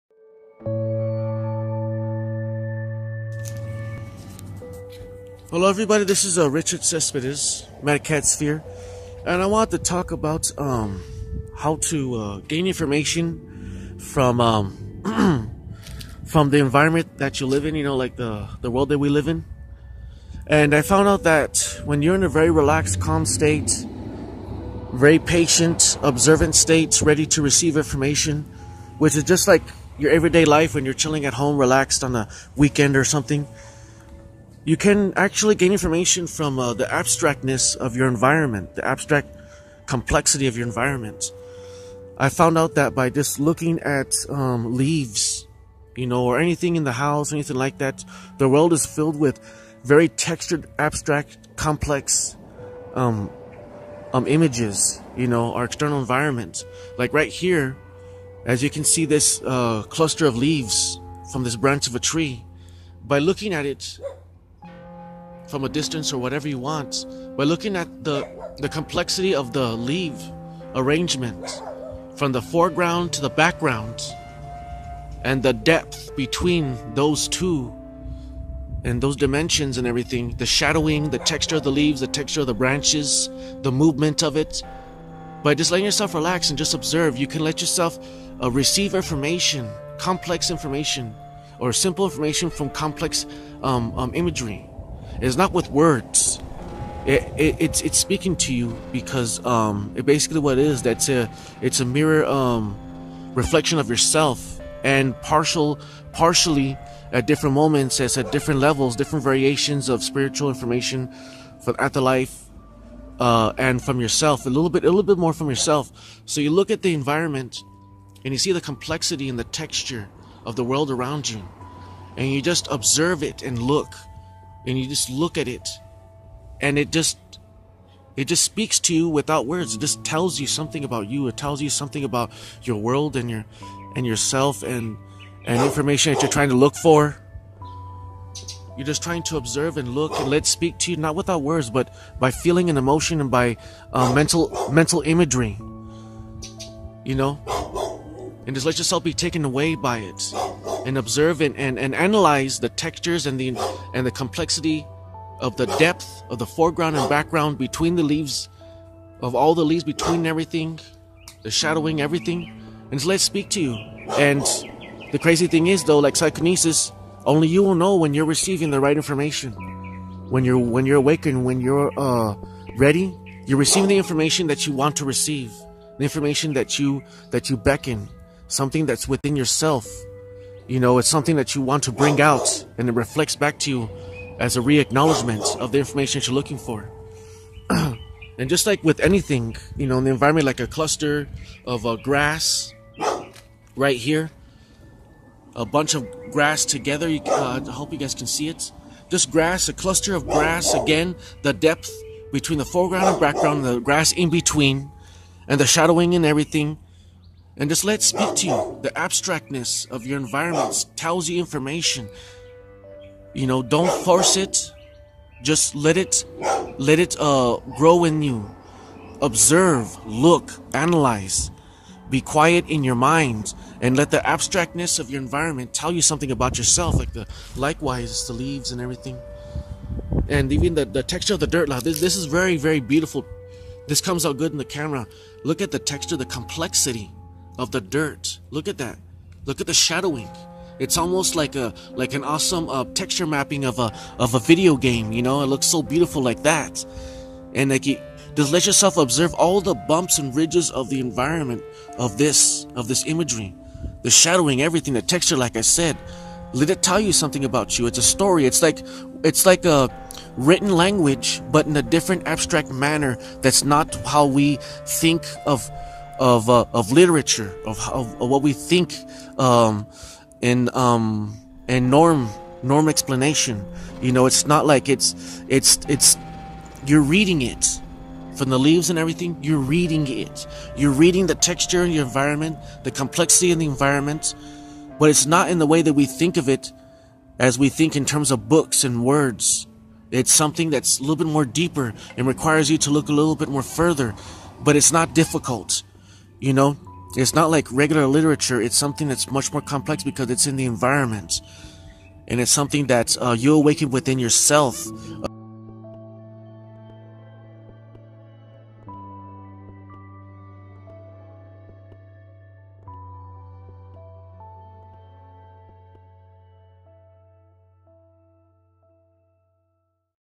How To Decode Psychic Information From Your Environment!
I had to share this bit of information to ya guys. Its how to receive, read and decode information in the environment that your in. Its very similar to how Voo Doo people look at a pile of bones and tell you your future.